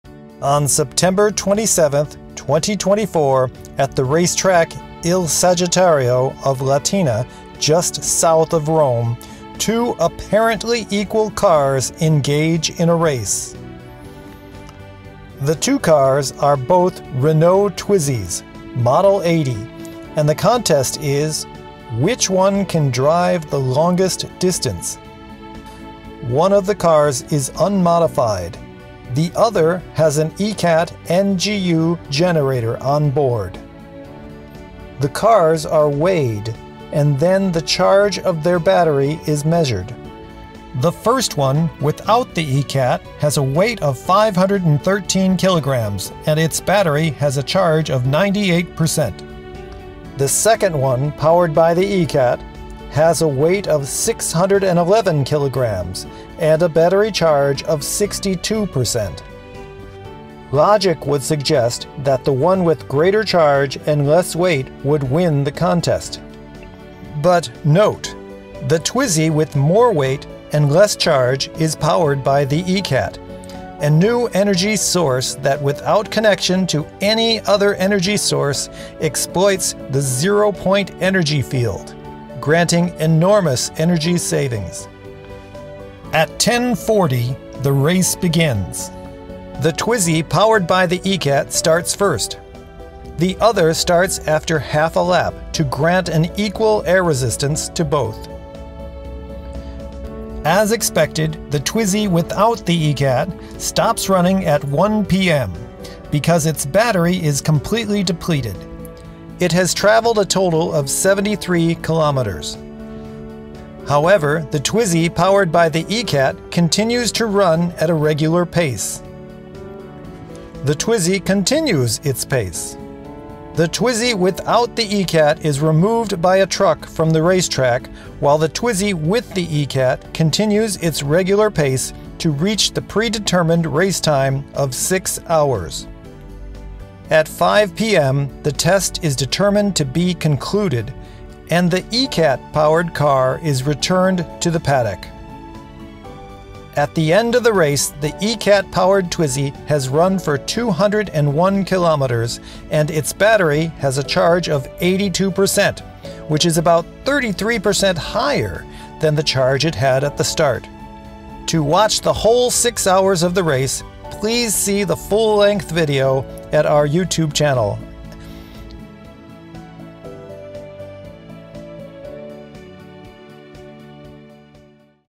Overview of the E-Cat EV Test in Latina, Italy (September 27th, 2024)
A short video explaining the E-Cat EV Test that took place on September 27th, 2024 in Latina, Italy.

Test protocol:

On September 27th, 2024, at the International Circuit of Latina, "Il Sagittario", racetrack in Latina, Italy, Leonardo Corporation held a presentation of its E-Cat SKLep NGU technology.

The expert responsible for validation was Engineer Maico Marzocchi, an independent specialist in electronic engineering, and an expert specifically of the Renault Twizy electric vehicle. The E-Cat used in this test was installed by him in his own Renault Twizy EV, independently from Andrea Rossi.

Also In attendance were: Andrea Rossi, CEO of Leonardo Corporation, Professors Guglielmo D’Inzeo, and Neri Accornero, both of the University La Sapienza of Rome.

CEOs and researchers from Italy, the USA, Germany, Japan, Canada and the United Kingdom were also in attendance. For security reasons, the attendance at the test was restricted to persons deeply involved in the activity, and the date and location have been kept confidential until now, when video documentation has been placed online. Access to the racetrack during the day of the test was protected by security professionals.

The presentation consisted of a comparison between two electric vehicles, both were the Renault Twizy 80. One of the cars had no modification, while the other car had installed in it an assembly of E-Cat power modules, along with associated electronic circuits that connected to the battery of the EV to allow for continuous charging.

The purpose of the test was to show that E-Cat technology can continuously charge the battery of an electric vehicle continuously while it is driving.

Prior to the test, the state of charge of each of the cars’ batteries was measured. The unmodified car had a starting battery charge of 96.00 per cent. The car with the E-Cat on board started with a charge of 62.18 per cent.

Also, prior to the test, each of the cars was weighed. The unmodified car weighed 513 kg, while the E-Cat car weighed 611 kg.

The difference in weight was due to the E-Cat and its associated equipment on board The E-Cat car. The weight can be broken down as follows:

The circuitry designed by engineer Maico Marzocchi  that connected the power produced by the E-Cat to the battery weighed 10 kg;

The E-Cat itself, and associated components weighed 8 kg

A Steel Box containing the E-Cat and its associated components weighed 80 kg.

The E-Cat and components were placed inside a fireproof and explosion-proof box as a necessary safety precaution to prevent possible damage if there were any explosion or fire.

The batteries of each car were identical, they were the original batteries of the Renault Twizy 80, each weighing 110 kg.

The two cars began the test at the same time 10:45 am, driving about 200 meters apart around an approximately 600 m circuit of the racetrack. The unmodified car  was driven by an employee of Leonardo Corporation. The E-Cat powered car was driven by Andrea Rossi and Maico Marzocchi, who traded driving duties. The only time the E-Cat car stopped during the test was for a few seconds every 90 minutes in order for drivers to be exchanged.

After 2 hours and 20 minutes, the unmodified car stopped running, due to its battery being fully depleted. It had driven 73 km in total. The E-Cat powered car continued to drive until 5.00 pm when it was determined that the test was concluded. This car had driven 201 km.

At the end of the test, the state of charge (SOC) of each car’s battery was taken again. The unmodified car had a charge of 0 per cent. The E-Cat powered car had a state of charge of 83 per cent, which is 33 per cent higher than when the test started.

We can conclude from the test that E-Cat technology has the ability to maintain a charge in a battery, and even increase it over time, without being connected to any external fuel or energy source.

Read our white paper: "E-Cat Power : The Impossible Made Possible"

Andrea Rossi's paper "E-Cat SK and Long-range Particle Interactions" (Theoretical explication of the science of the E-Cat)